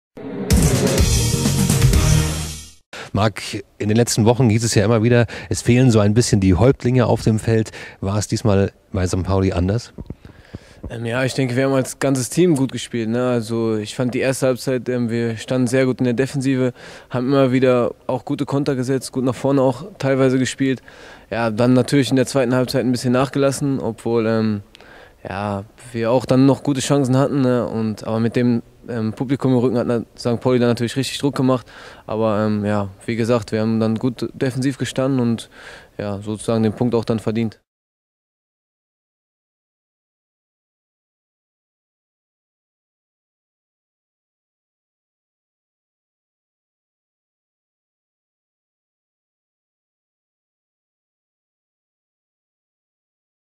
"Ich fühle mich wohl"
Immer besser in Tritt, immer präsenter auf dem Platz. Marc-André Kruska ist angekommen beim FC Energie. Im Spiel gegen St. Pauli einer der Besten freut er sich jetzt auf das Duell in Bielefeld. Der Mann des Spiels auf FCEnergie.tv